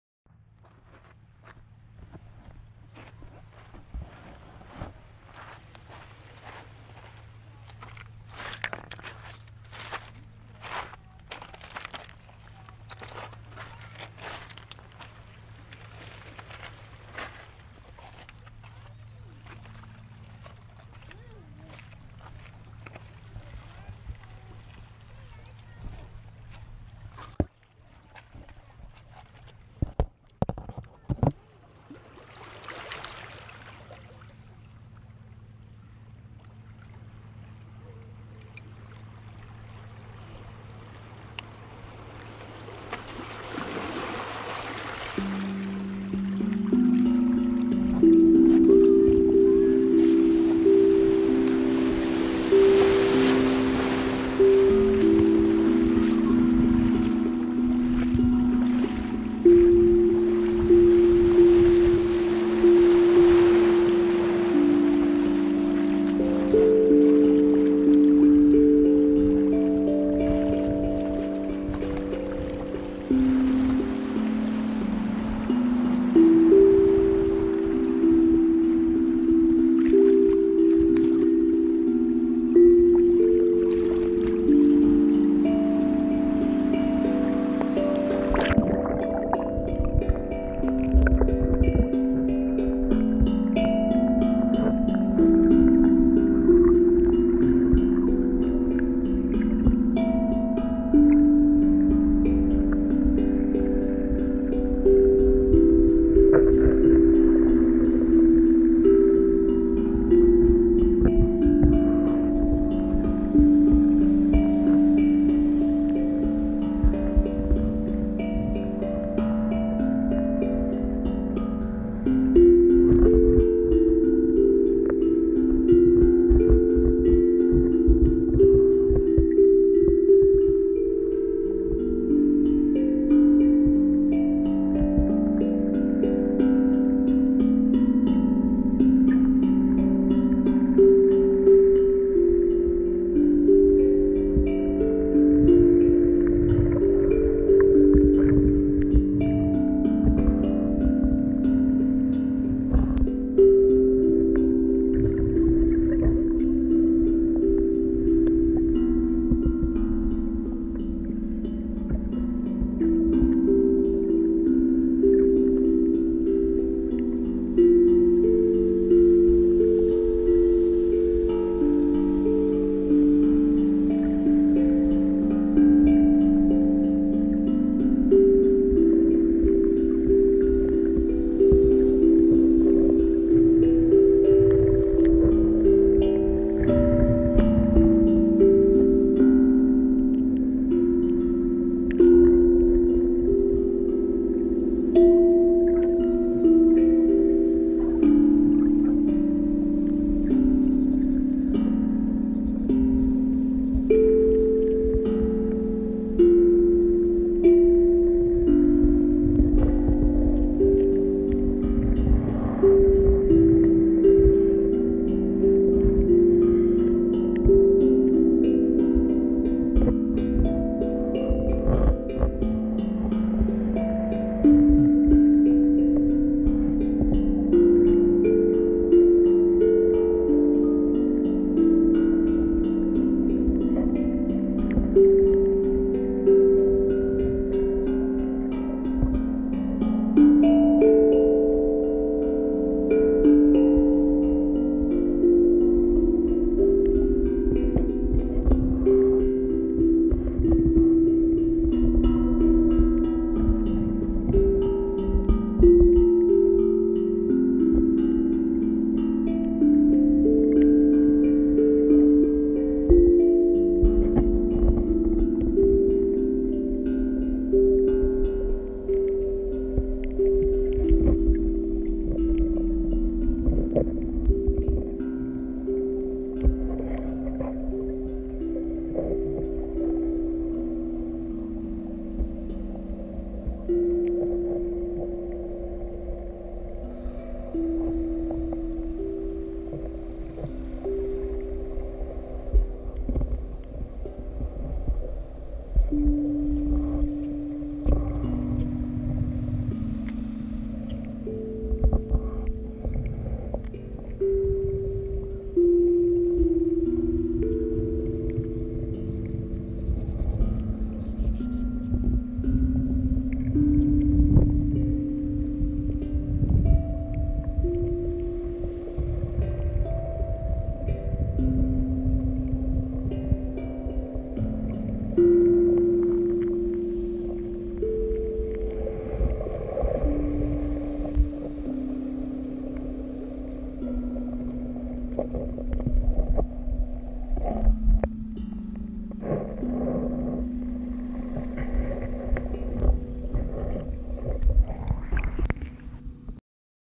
Float
A short snorkeling visit with hang drum accompaniment